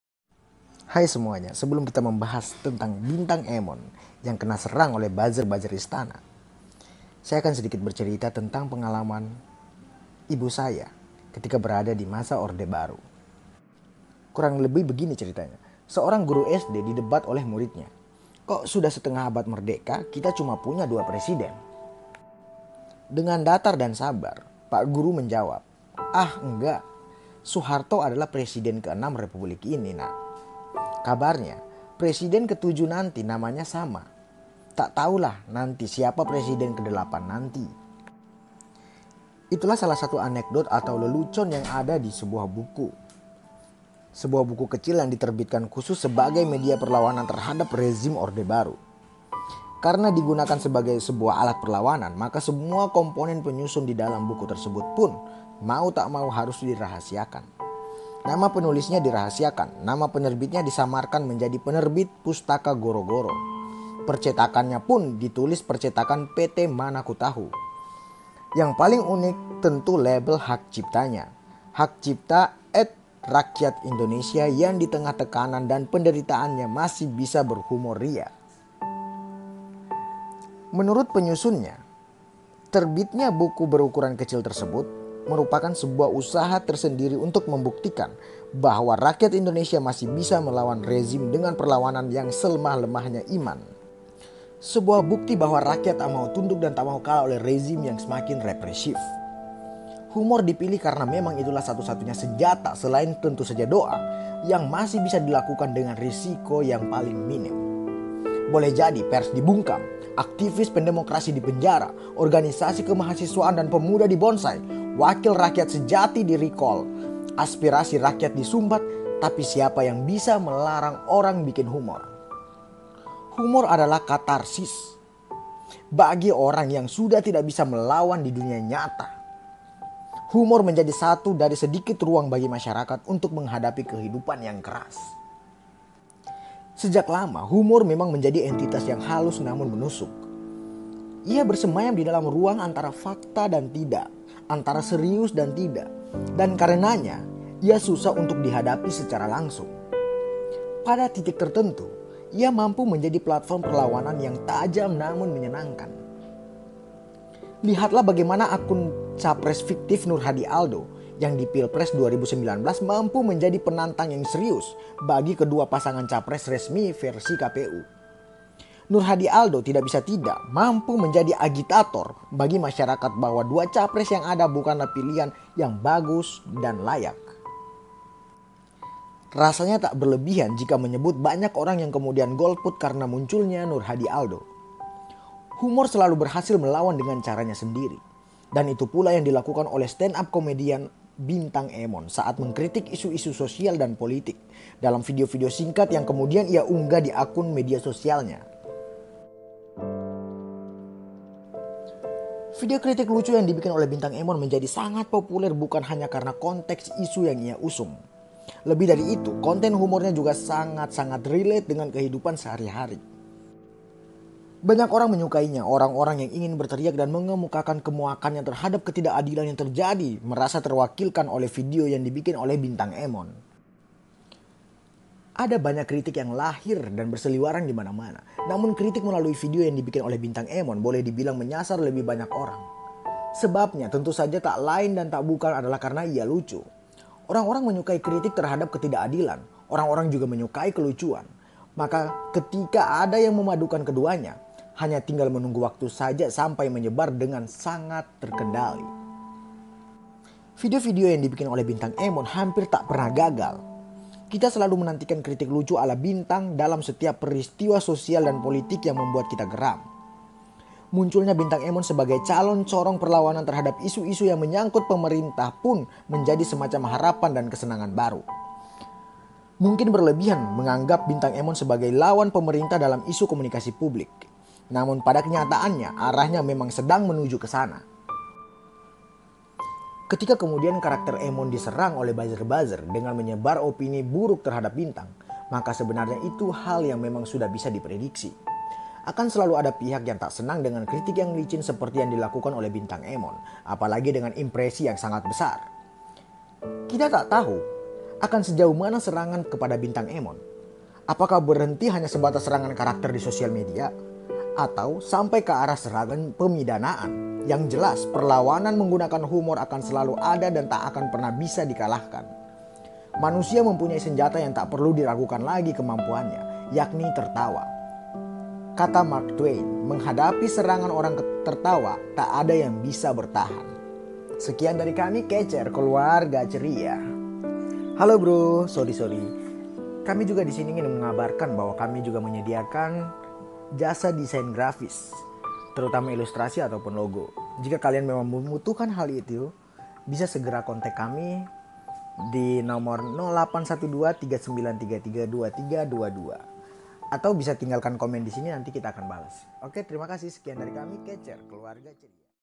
Bintang Emon Mungkin bisa Diserang Karakternya, tapi tidak dengan HUMORNYA
HUMOR ADALAH KATARSIS BAGI MEREKA YG MERASA TAK BISA MELAWAN SECARA LANGSUNG.

Sejak lama, humor memang menjadi entitas yang halus namun menusuk. Ia bersemayam di dalam ruang antara fakta dan tidak. Antara serius dan tidak. Dan karenanya, ia susah untuk dihadapi secara langsung.

Pada titik tertentu, ia mampu menjadi platform perlawanan yang tajam namun menyenangkan.

Lihatlah bagaimana pengelola akun-akun agama garis lucu mencoba mendobrak hegemoni keberagamaan yang entah kenapa terasa begitu kaku dan keras. Akun-akun agama garis lucu itu, membawa kelenturan dan kelucuan dari agama dan alirannya masing-masing. Dan itu menyenangkan.

Ia seakan mampu menjadi lawan yang tangguh bagi para pemeluk agama yang “garis keras”.

Atau lihatlah bagaimana akun capres fiktif Nurhadi-Aldo yang di Pilpres 2019 mampu menjadi penantang yang serius bagi dua pasangan capres resmi versi KPU. Nurhadi-Aldo, tak bisa tidak, mampu menjadi agitator bagi masyarakat bahwa dua capres yang ada bukanlah pilihan yang bagus dan layak.

Rasanya tak berlebihan jika menyebut banyak orang yang kemudian golput karena munculnya Nurhadi-Aldo.

Humor selalu berhasil melawan dengan caranya sendiri. Dan itu pula yang dilakukan oleh stand up comedian Bintang Emon saat mengkritik isu-isu sosial dan politik dalam video-video singkat yang kemudian ia unggah di akun sosial medianya.

Video kritik lucu yang dibikin oleh Bintang menjadi sangat populer bukan hanya karena konteks isu yang ia usung, lebih dari itu, konten humornya juga segar dan sangat relate dengan kehidupan sehari-hari.

Banyak orang menyukainya. Orang-orang yang ingin berteriak dan mengemukakan kemuakannya terhadap ketidakadilan yang terjadi merasa terwakilkan oleh video yang dibikin oleh Bintang Emon.

Ada banyak kritik yang lahir, namun kritik melalui video yang dibikin oleh Bintang Emon boleh dibilang menyasar lebih banyak orang. Sebabnya tentu saja tak lain dan tak bukan adalah karena ia lucu. Orang-orang menyukai kritik terhadap ketidakadilan, orang-orang juga menyukai kelucuan, maka ketika ada yang memadukan keduanya, hanya tinggal menunggu waktu sampai ia menyebar dengan sangat terkendali.

Video-video yang dibikin oleh Bintang Emon hampir tak pernah gagal.

Kita selalu menantikan kritik lucu ala bintang dalam setiap peristiwa sosial dan politik yang membikin kita geram.

Munculnya Bintang Emon sebagai “calon” corong perlawanan terhadap isu-isu yang menyangkut pemerintah pun menjadi semacam harapan dan kesenangan baru. Mungkin berlebihan menganggap Bintang sebagai lawan pemerintah dalam isu komunikasi publik. Namun pada kenyataannya, arahnya memang sedang menuju ke sana.

Ketika kemudian karakter Bintang Emon diserang oleh buzzer-buzzer dengan menyebarkan opini buruk terhadap Bintang, maka sebenarnya itu hal yang memang sudah bisa diprediksi. Akan selalu ada pihak yang tak senang dengan kritik yang licin seperti yang dilakukan oleh Bintang Emon, apalagi dengan impresi yang sangat besar.

Kita tak tahu, akan sejauh mana serangan kepada Bintang, apakah berhenti hanya sebatas serangan karakter di sosial media atau sampai ke arah serangan “pemidanaan”, yang jelas, perlawanan menggunakan humor akan selalu ada dan tak akan pernah bisa dikalahkan.

“Manusia mempunyai senjata yang tak perlu diragukan lagi keampuhannya, yakni tertawa,” kata Mark Twain, “Menghadapi serangan orang tertawa, tak ada yang bisa bertahan.”